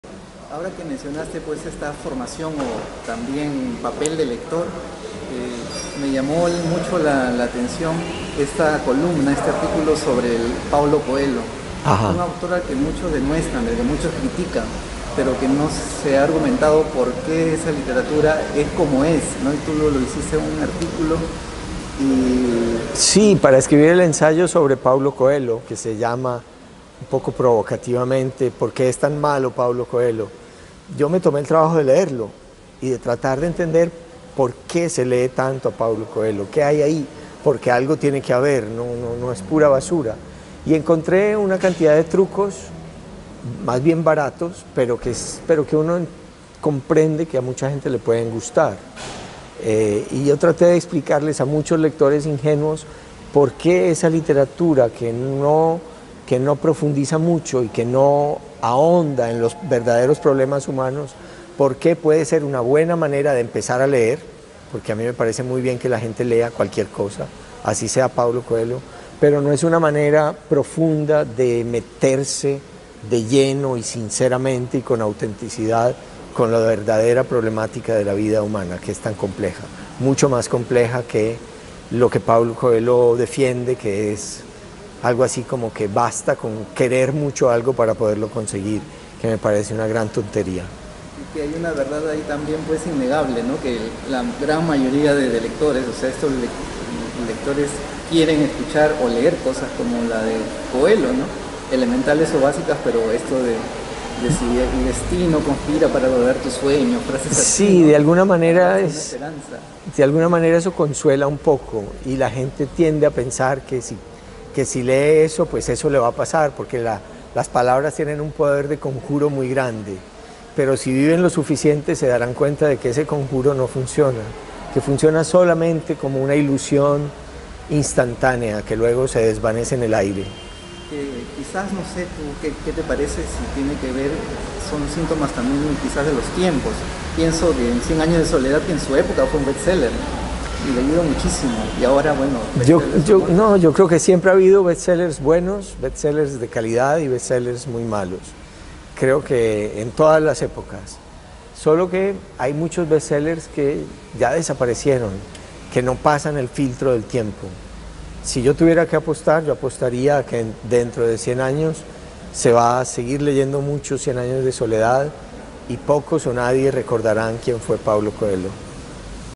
Héctor Abad Faciolince y por qué es tan malo Paulo Coelho
A propósito de un artículo suyo titulado «¿Por qué es tan malo Paulo Coelho?», el escritor colombiano Héctor Abad Faciolince nos comparte aquí sus razones.